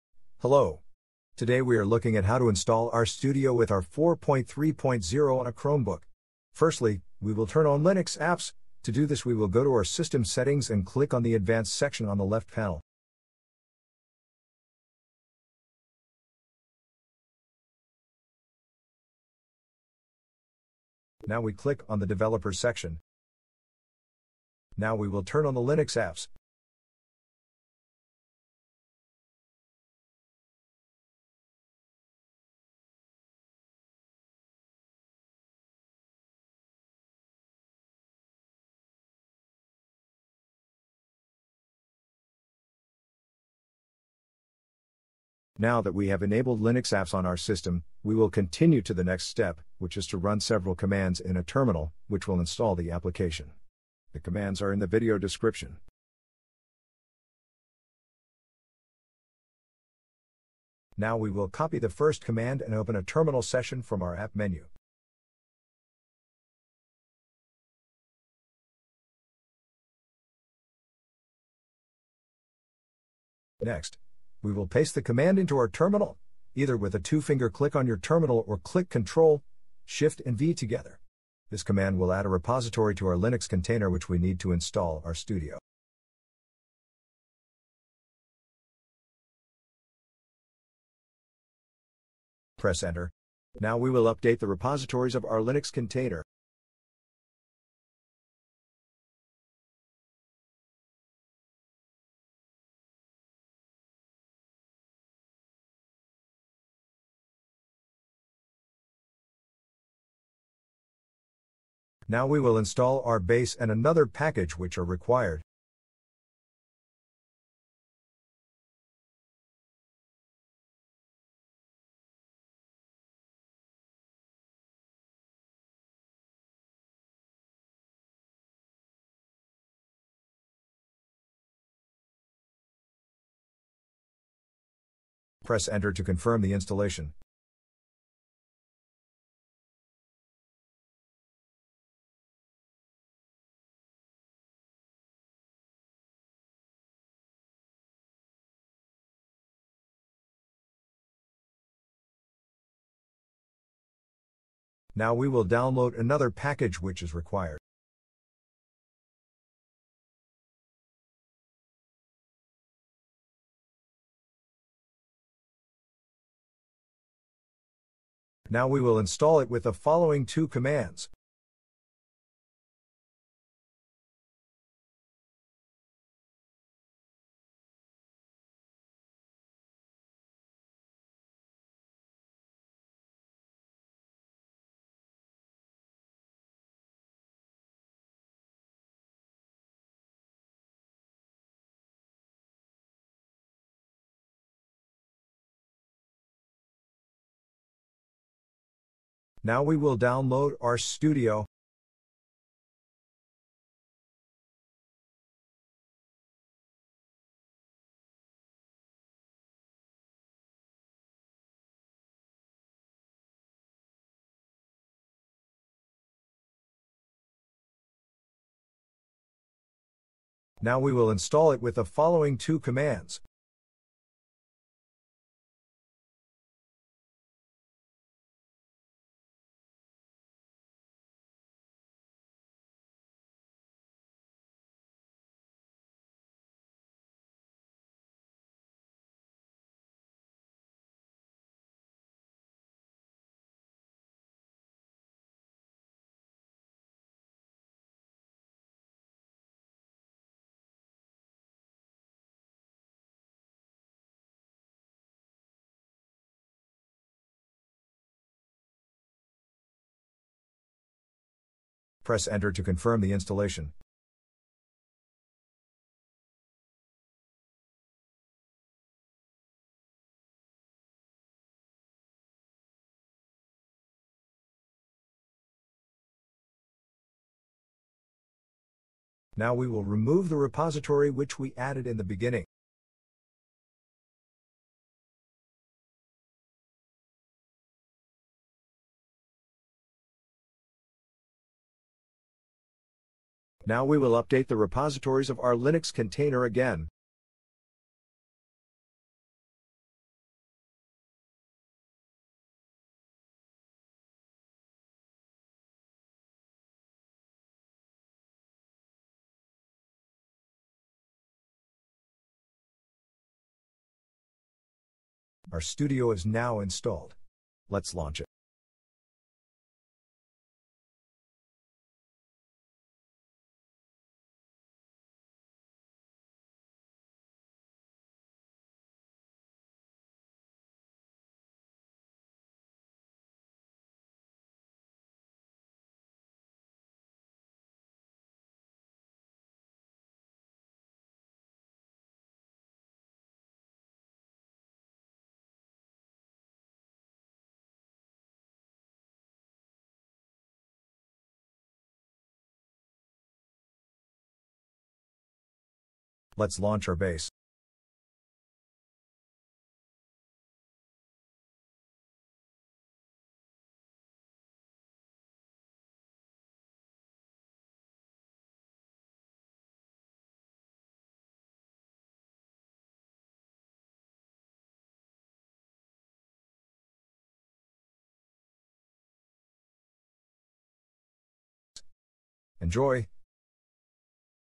How to install RStudio and R 4.3.1 on a Chromebook
In this video, we are looking at how to install RStudio and R 4.3.1 on a Chromebook.